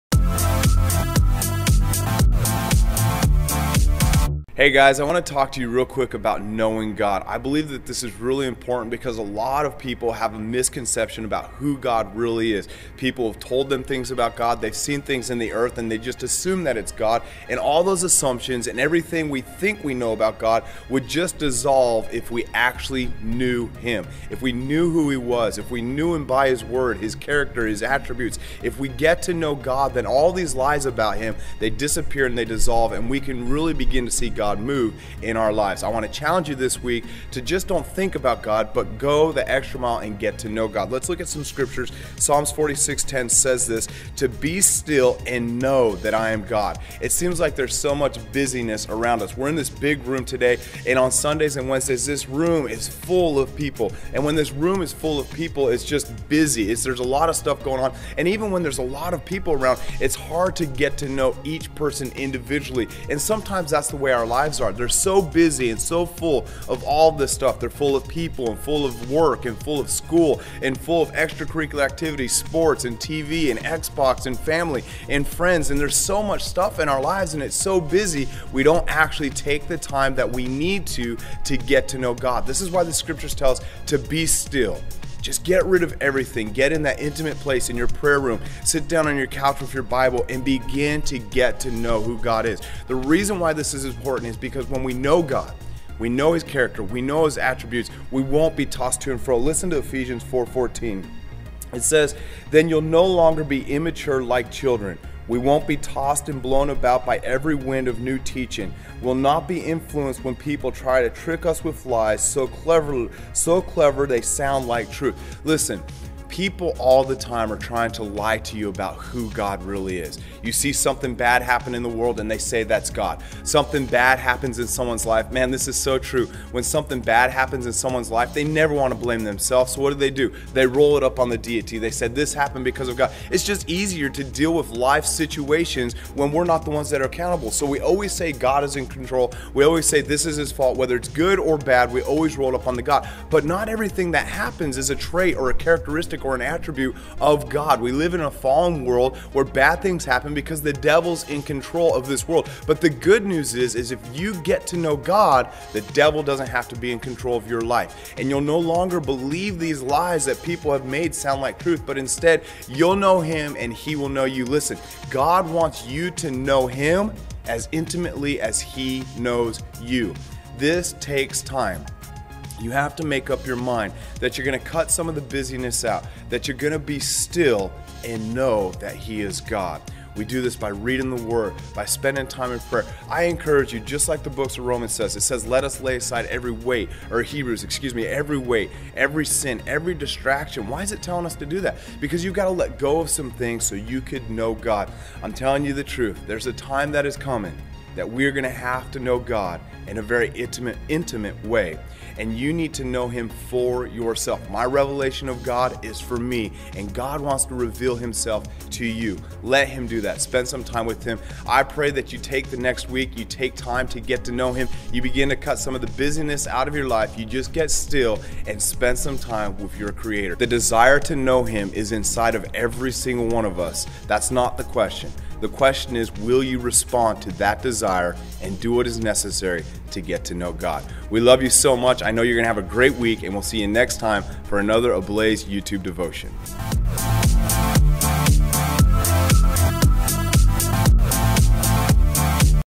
WEAREABLAZE - To Know
Pastor Robert speaks on the importance of knowing the character of God.